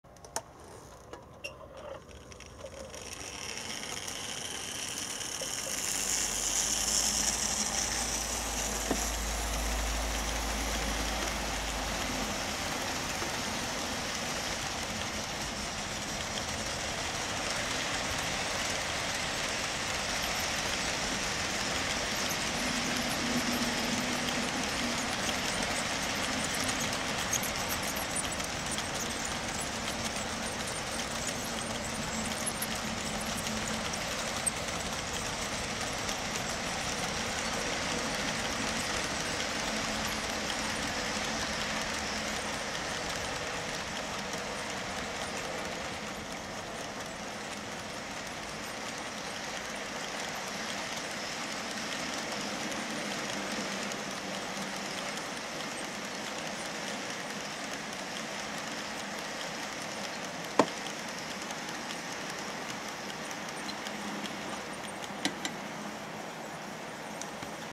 BR 44 als 3047 mit 2 HR vor leerem "Langen Heinrich"
44 690 zieht den "Langen Heinrich" ohne Probleme an und zieht gemächlich ihre Runden. Der Zug besteht aus 50 Fads-Wagen von Märklintyp 4624 alt und neu. Erzielte Geschwindigkeit: 56,06s für 6,7m = 0,120 m/s im Modell, entspricht ca. 37 km/h. Leistungsabschätzung:
P = m*g*v = 88g (Zugmasse) * 9,81 * 0,12 = ca. 104mW